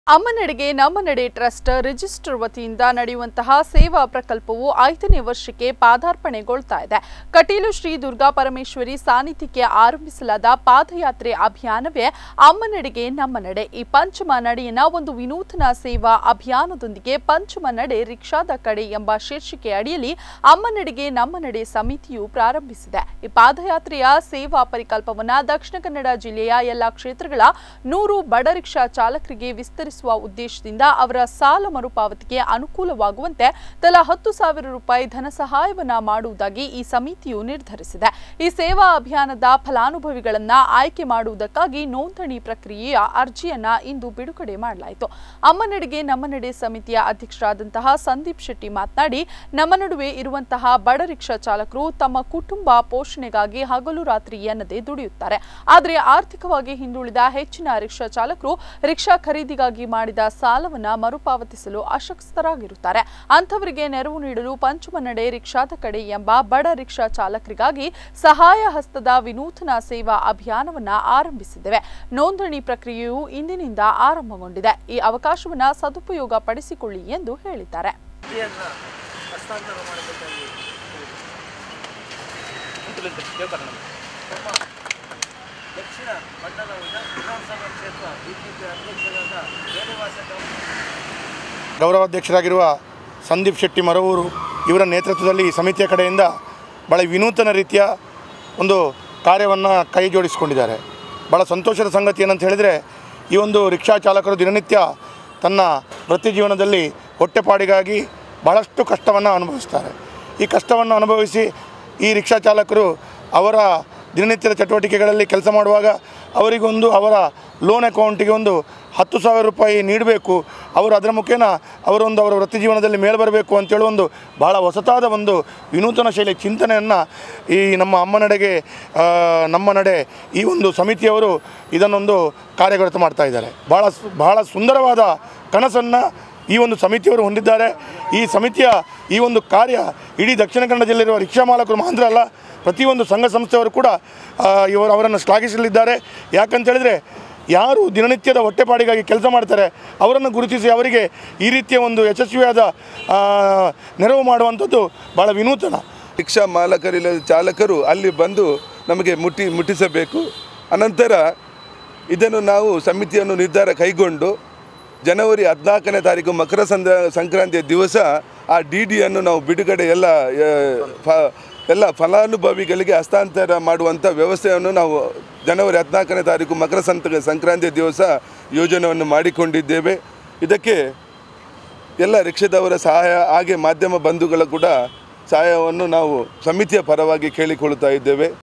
ಅಮ್ಮನೆಡೆಗೆ ನಮ್ಮ ನಡೆ ಸಮಿತಿ 5 ನೇ ವರ್ಷಕ್ಕೆ ಪಾದರ್ಪಣೆ
ಅಮ್ಮನೆಡೆಗೆ ನಮ್ಮ ನಡೆ ಸಮಿತಿ
5 ನೇ ವರ್ಷಕ್ಕೆ ಪಾದರ್ಪಣೆ
ದುರ್ಗಾ ಪರಮೇಶ್ವರಿ ಸಾನಿಧ್ಯಕ್ಕೆ ಆರಂಭಿಸಲಾದ ಪಾದಯಾತ್ರೆ
ಪಂಚಮ ನಡೆ ವಿನೂತನ ಸೇವಾ ಅಭಿಯಾನ